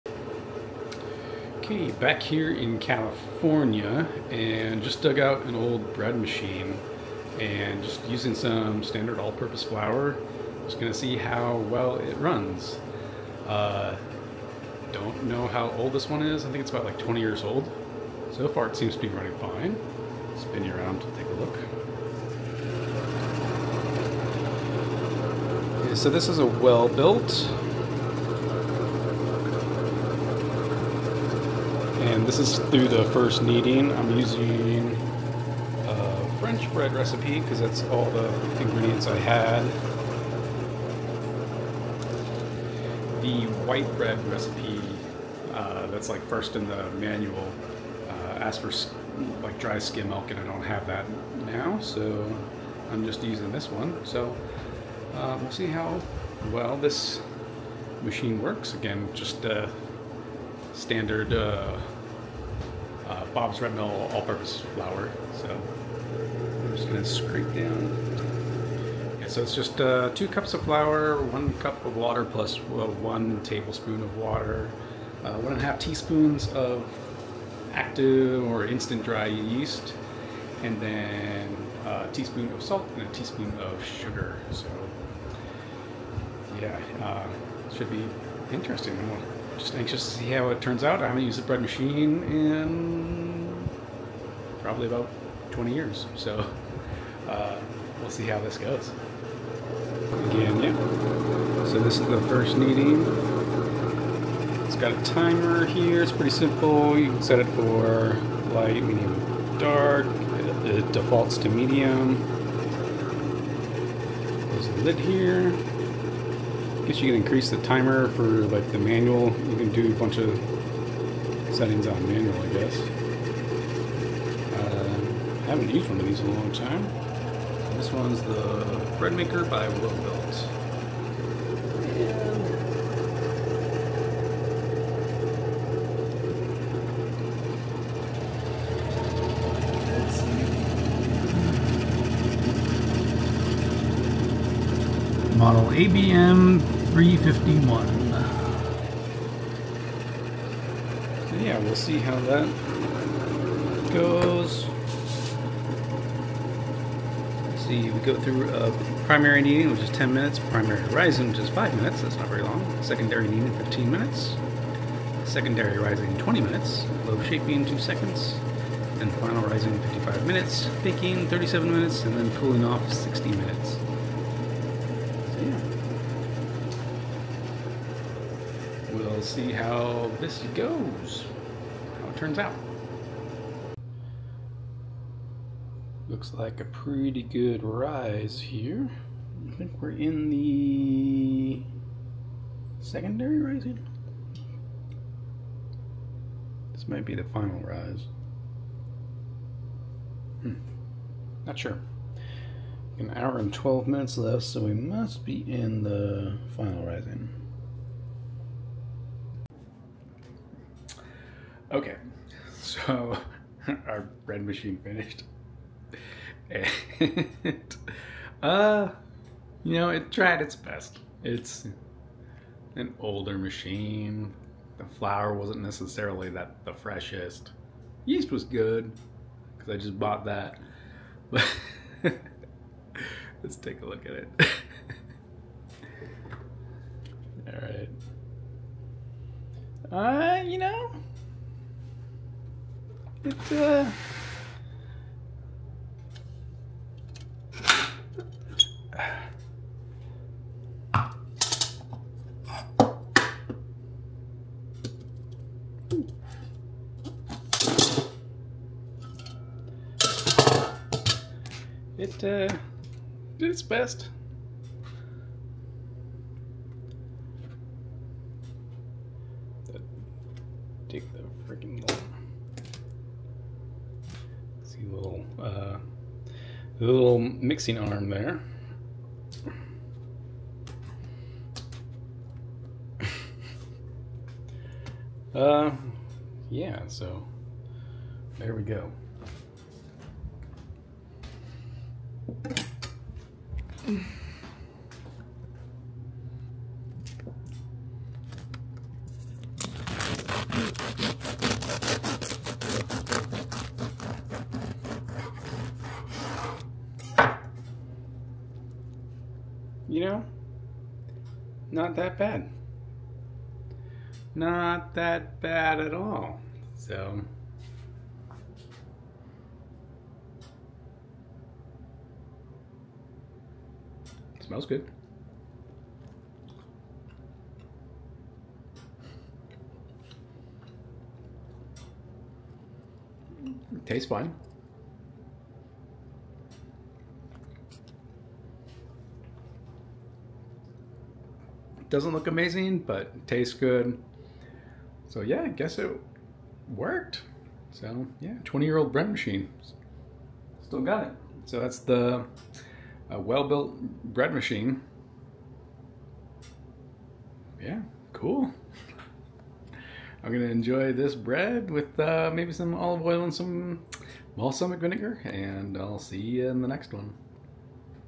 20+ Year Old Bread Machine Surprises Me!
I found a 20+ year old bread machine at my place in California and decided to give a try! The results surprised me! The bread looks weird but tastes delicious! The machine is a Welbilt Bread Machine.
If you’ve got an old bread maker sitting in your cupboard, don’t let it collect dust—bring it back to life and start baking fresh, fluffy bread right at home.